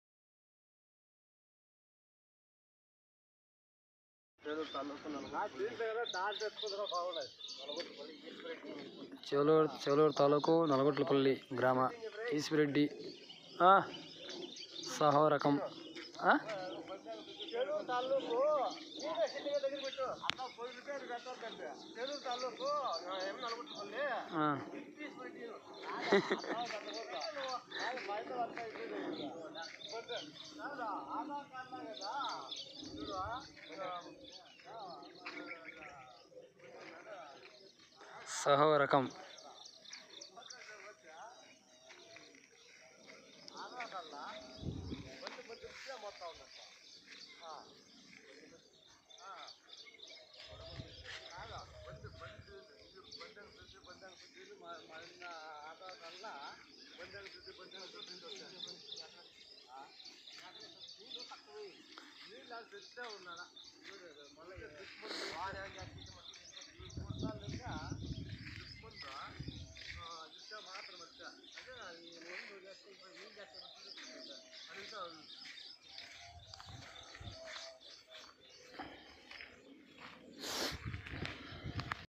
15-07-2023 కొత్త టమోటా తోట ఇన్ఫర్మేషన్ మీ కోసం రైతన్న మాటల్లో/new tomato crops information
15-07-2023 కొత్త టమోటా తోట ఇన్ఫర్మేషన్ మీ కోసం రైతన్న మాటల్లో/new tomato crops information